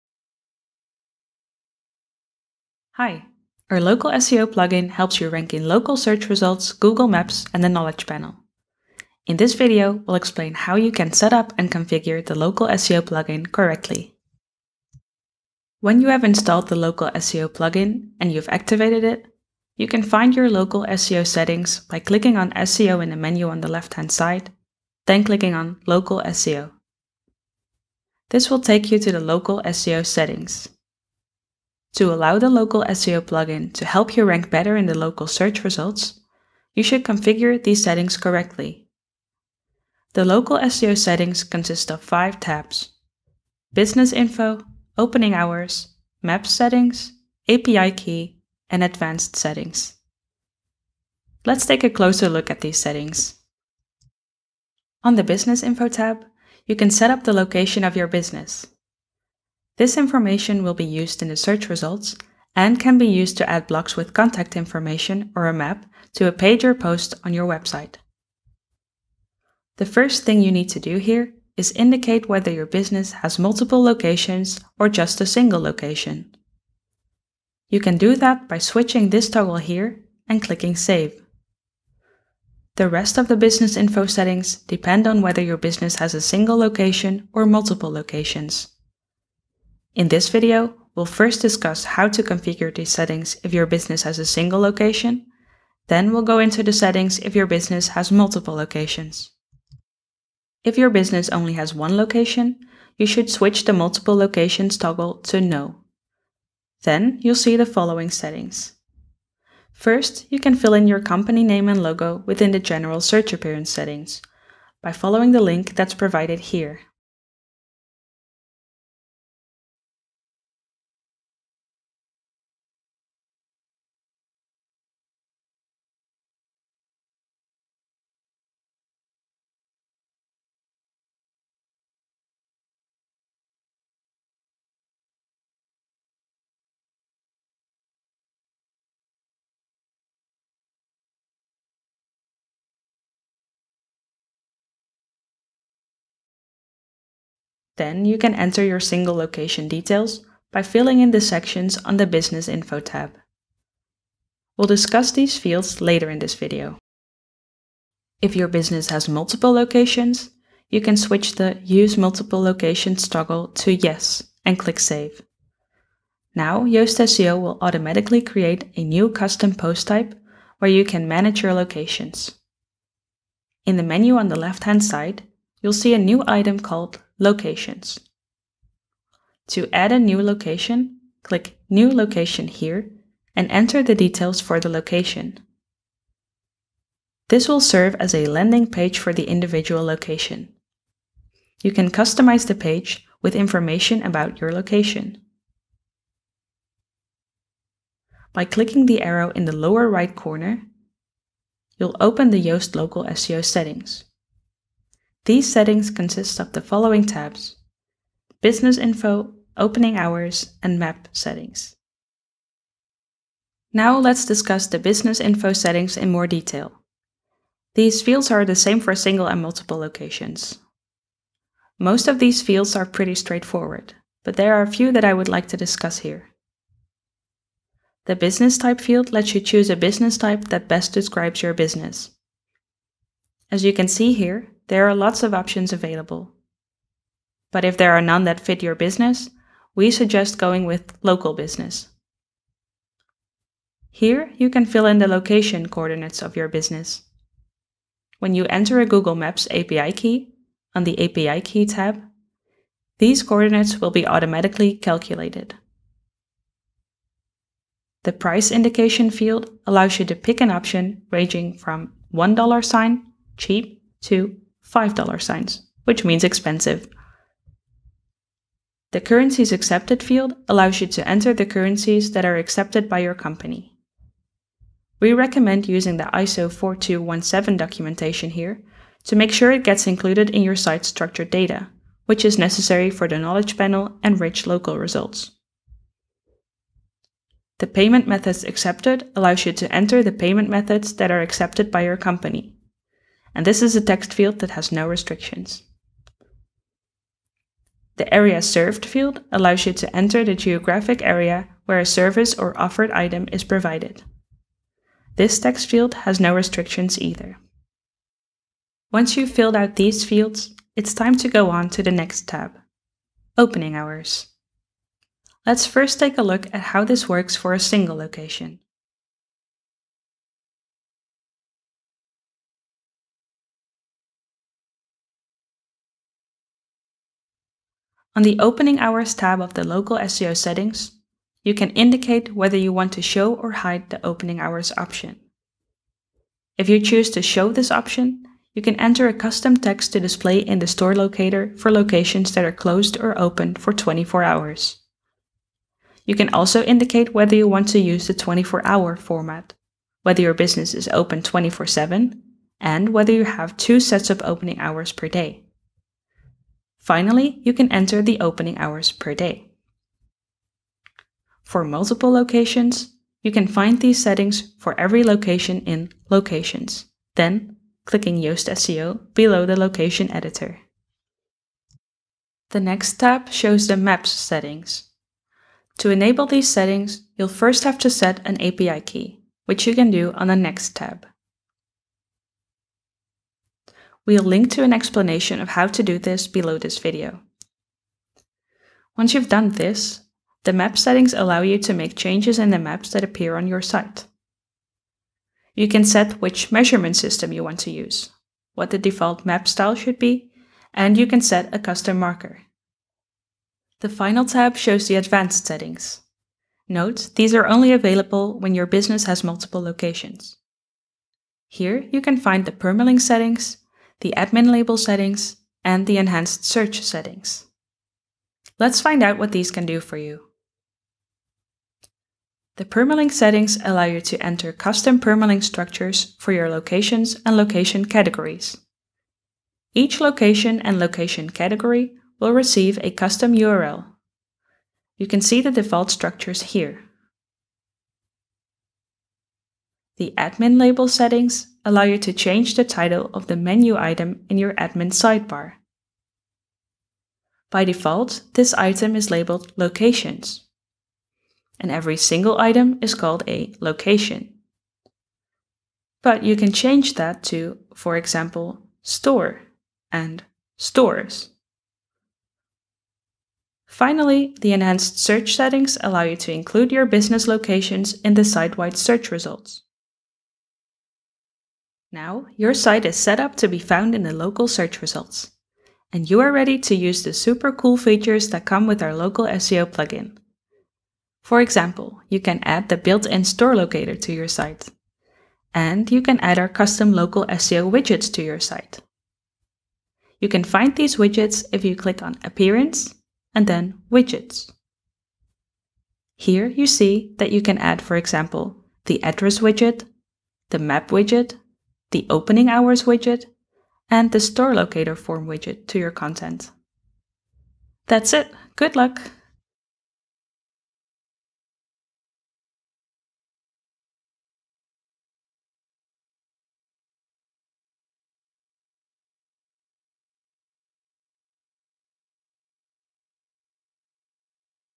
How to configure the Local SEO plugin | Yoast Local SEO
This video explains how to configure the Yoast Local SEO plugin. Find out more about Yoast Local SEO

Our Local SEO plugin helps you rank in local search results, Google Maps, and the Knowledge Panel. This video helps you configure your Local SEO plugin correctly.

In Yoast Local SEO 13.9, we've released new features!